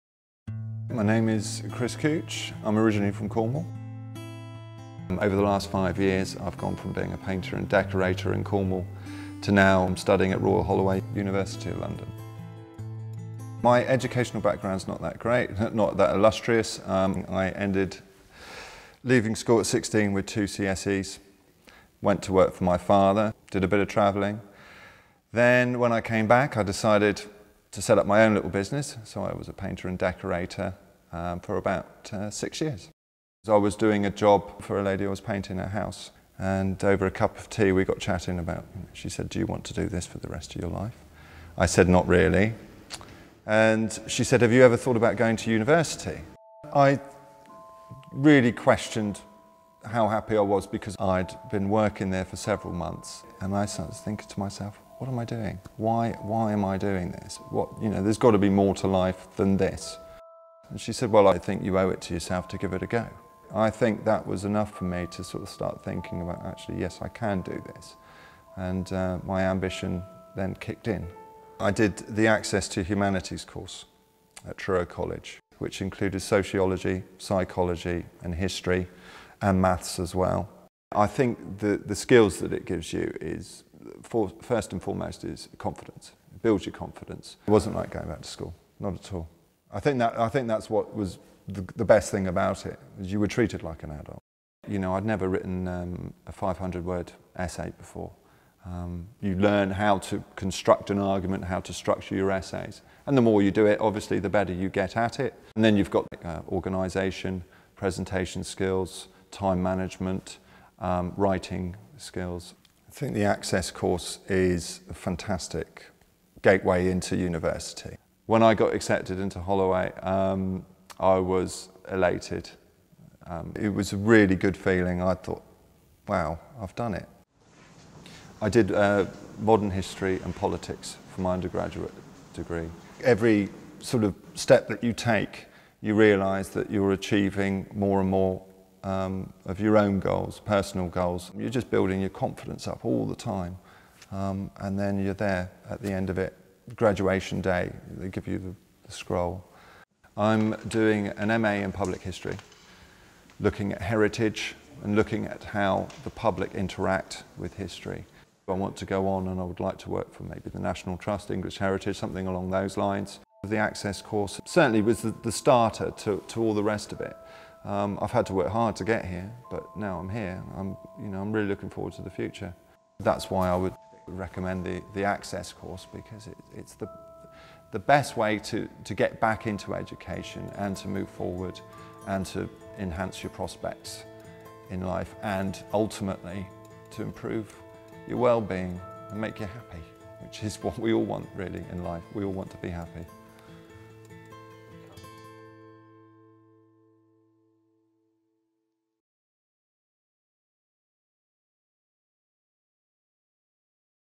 Access to HE - Chris Couch
From being a painter and decorator in Cornwall to studying an MA at Royal Holloway, University of London, Chris Couch describes his experience of Access to HE.